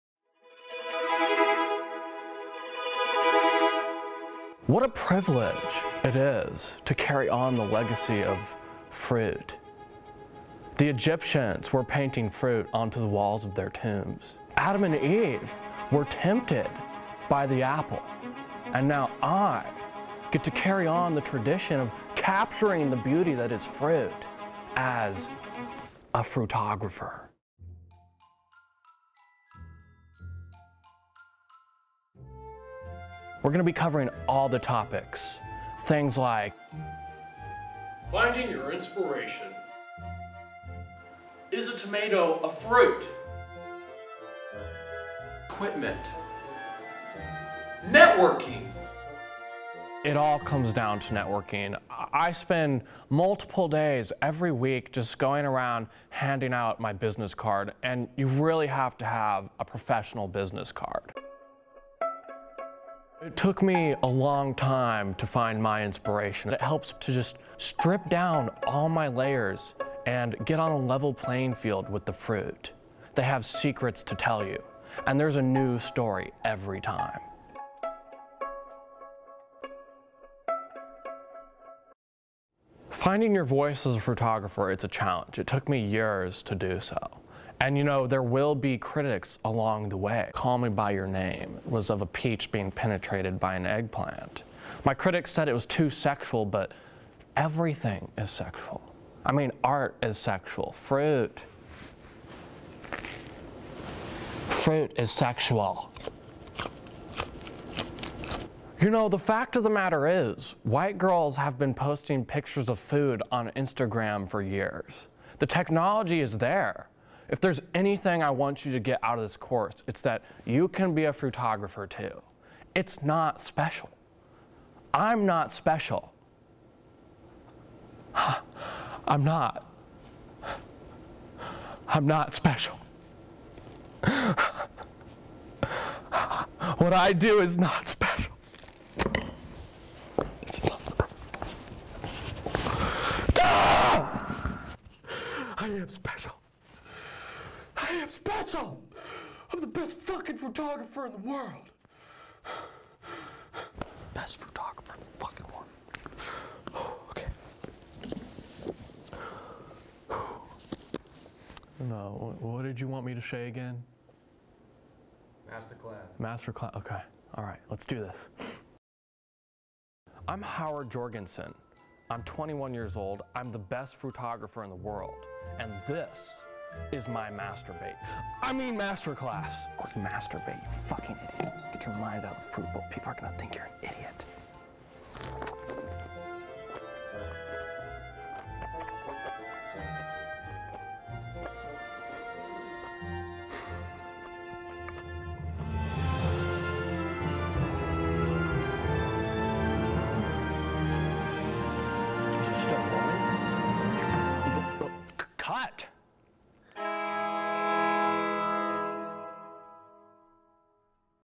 How to Take Picture of Fruit
A comedy spoof of masterclass tutorial trailers and over-pretentious art.

2018 Tensie Nomination for Best Comedy, Best Editing, Best Experimental Project, and Best Screenplay

Produced for FMS 302/THR 506, Laura Kirk, Sp18

Director
Erik Harken

Cinematography
Gary Lange

Editor
Gary Lange

Sound
Erik Harken

Screenwriter
Erik Harken

Visual Effects
Gary Lange